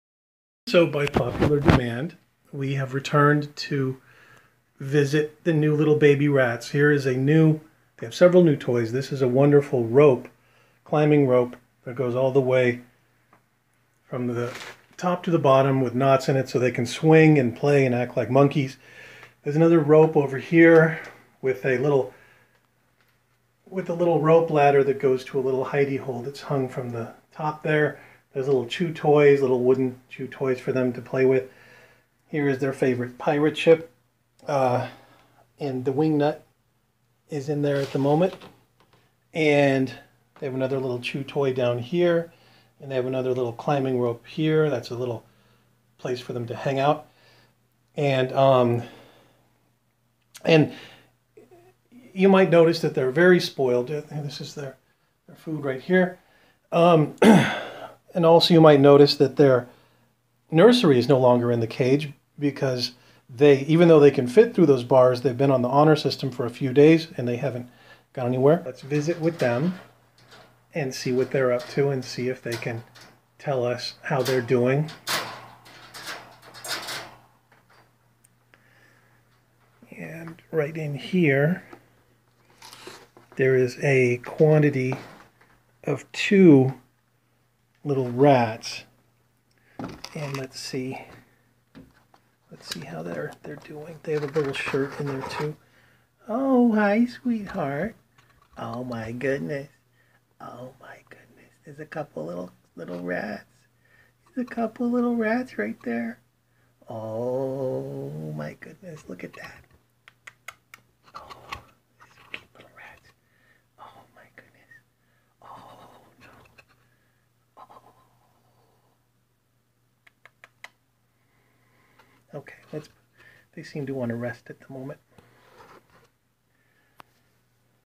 My New Baby Rats: Day 6
They had just feasted on a breakfast burrito and wanted nothing more than to rest, but their adoring fans were clamoring for an appearance. They taught me a new game. I will film it for the next movie.